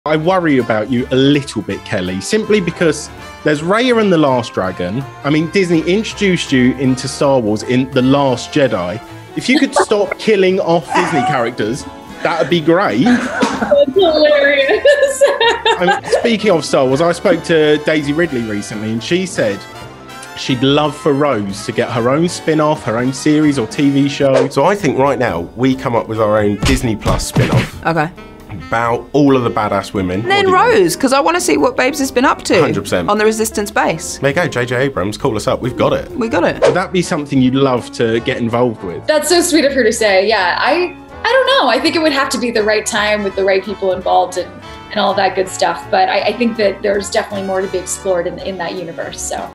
Kelly Marie Tran To Reprise Rose Tico In Star Wars? 🚀 | The Movie Dweeb
Daisy Ridley said she'd be keen to see Rose Tico get her own Disney+ spin-off series, and Kelly Marie Tran said that - with the right cast and crew - she may consider returning to Star Wars.

Get more movie news, interviews and reviews over at The Movie Dweeb